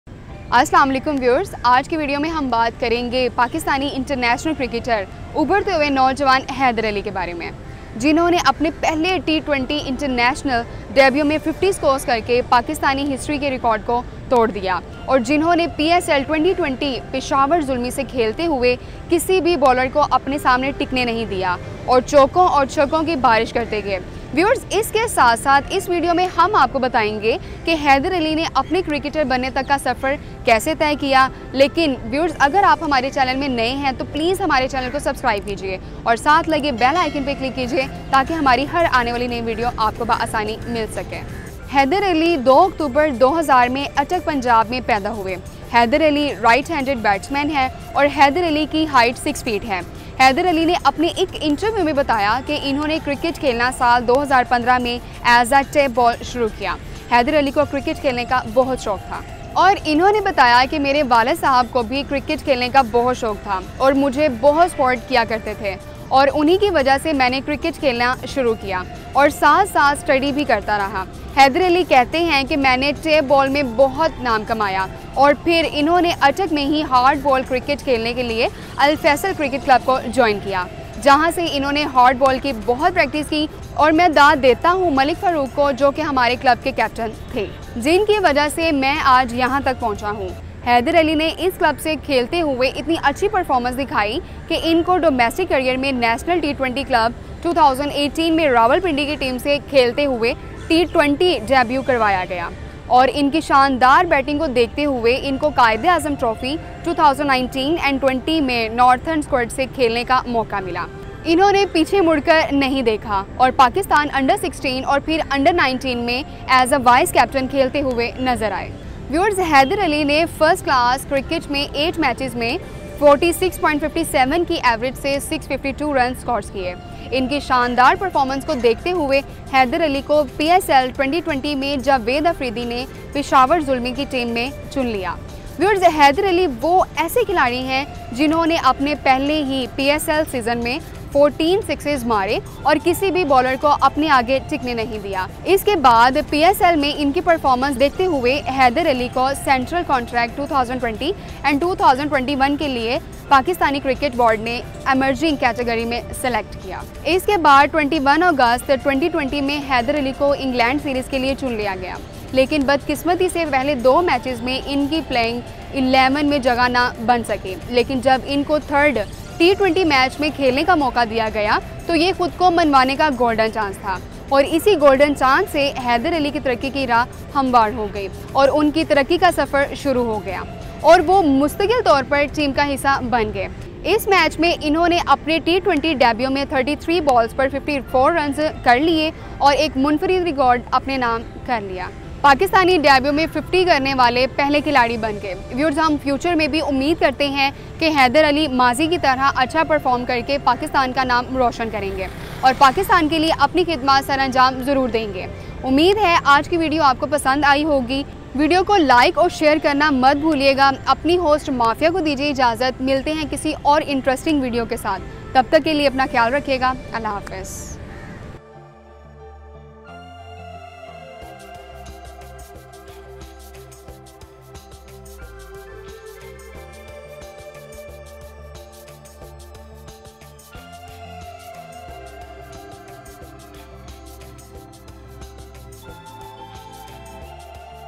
Haider Ali Biography | Cricket Career | Life Story | Celebrity Historia
About Haider Ali Biography:-
Haider Ali is a Pakistani cricketer. He made his first-class debut in September 2019. He made his international debut for the Pakistan cricket team on 1 September 2020
Haider Ali Born:                    2 October 2000 Attock , Punjab, Pakistan
Haider Ali Height:                 6 ft 0[1] in (1.83 m)
Haider Ali Batting:                Right-handed
Haider Ali Role:                     Batsman
Haider Ali Shirt No:              87
Haider Ali T20I Debut          1 September 2020 v England